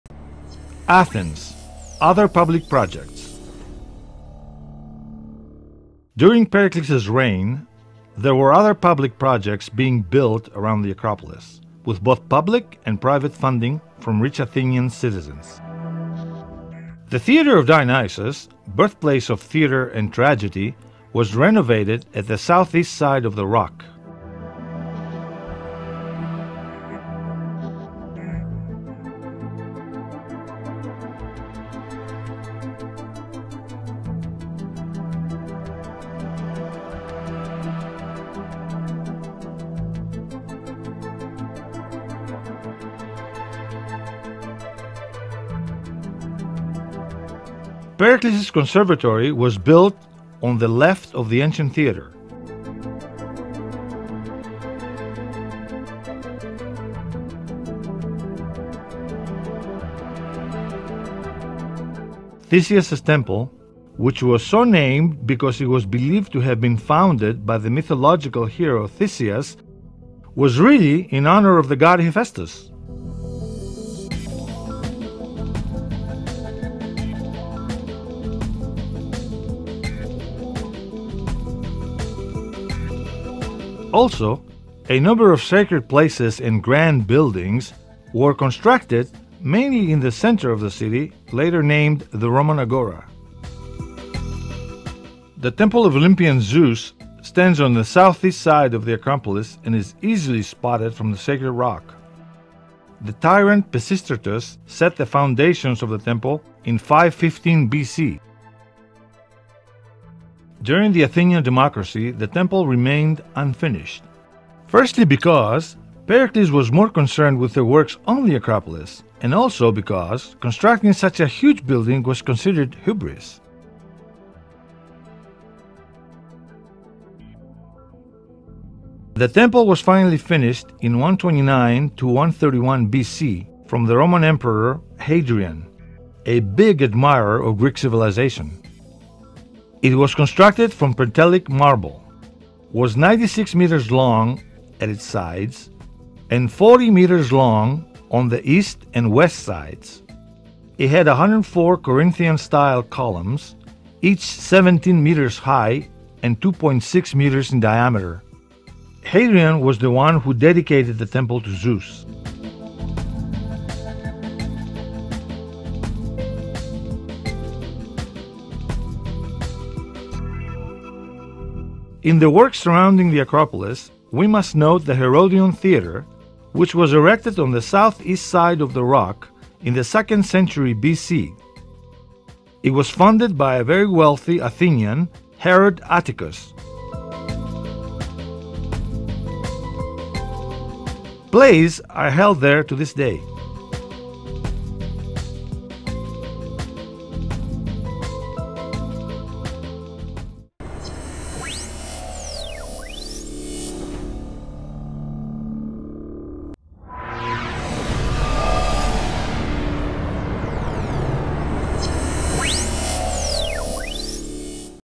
Ancient monuments in Athens (3D)
This video presents some famous buildings around acropolis of Athens, that was constructed during the Athenian democracy (500 BC), in 3D stereoscopic type (you need red-cyan glasses).
English tranlation Natalie Margarites, and voice over in English Kostas Gus Georgilas.
A video in 3D stereoscopic type (you need red-cyan glasses).

Research and Writing in the Greek language Eleftherios Katsaros
English translation Natalie Margarites, and voice over in English Gus Georgilas.
A movie by EleftheriosK.